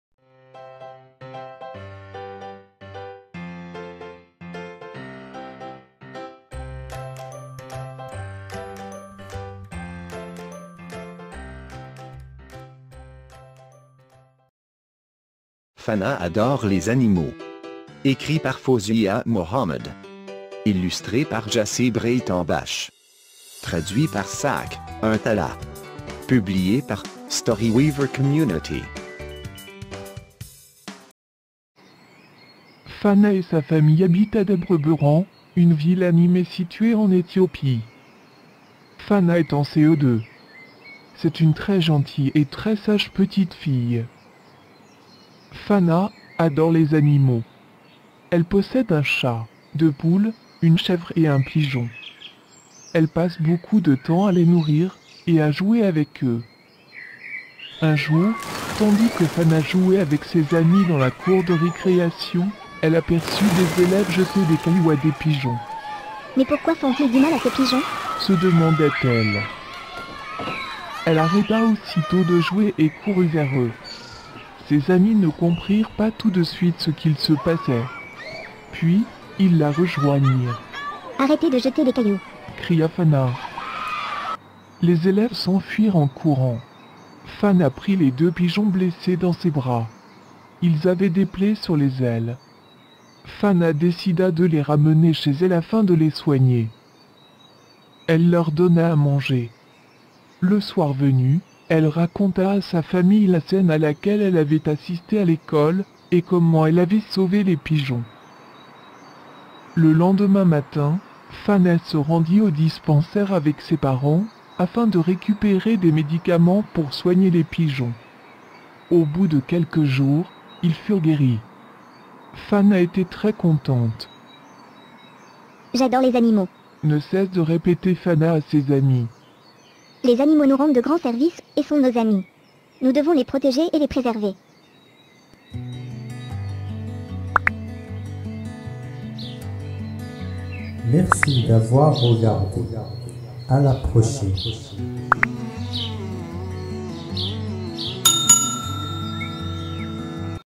🙂🐐🐓💓Fana adore les animaux💓🐐🐓🙂 | Histoire Pour S'endormir |Histoire Pour Les Petits| 4levelstories
Ici, nous présentons "Histoires pour les tout-petits | Histoires morales pour les enfants | Histoire traditionnelle |
✿‿✿ BONJOUR ENFANTS que vous appréciez "Fana Adore Les Animaux" par 4Levelstories.
💓Fana adore les animaux. Elle passe du temps avec eux, elle leur donne à manger et joue avec eux.💓
Fana adore les animaux
Author: Foziya Mohammed.
Illustrator: Jesse Breytenbach.
Translator: Sak Untala.
Fana et sa famille habitent à Debre Berhan, une ville animée située en Éthiopie.
Fana est en CE2. C'est une très gentille et très sage petite fille.
Fana adore les animaux. Elle possède un chat, deux poules, une chèvre et un pigeon.
Elle passe beaucoup de temps à les nourrir et à jouer avec eux.
Un jour, tandis que Fana jouait avec ses amis dans la cour de récréation, elle aperçut des élèves jeter des cailloux à des pigeons.
Mais pourquoi font-ils du mal à ces pigeons ?.se demanda-t-elle.
Elle arrêta aussitôt de jouer et courut vers eux. Ses amis ne comprirent pas tout de suite ce qu'il se passait. Puis, ils la rejoignirent.
« Arrêtez de jeter des cailloux ! » cria Fana.
Les élèves s'enfuirent en courant. Fana prit les deux pigeons blessés dans ses bras.
Ils avaient des plaies sur les ailes. Fana décida de les ramener chez elle afin de les soigner.
Elle leur donna à manger.
Le soir venu, elle raconta à sa famille la scène à laquelle elle avait assisté à l'école et comment elle avait sauvé les pigeons.
Le lendemain matin, Fana se rendit au dispensaire avec ses parents afin de récupérer des médicaments pour soigner les pigeons.
Au bout de quelques jours, ils furent guéris.
Fana était très contente.
« J'adore les animaux, ne cesse de répéter Fana à ses amis. Les animaux nous rendent de grands services et sont nos amis. Nous devons les protéger et les préserver. »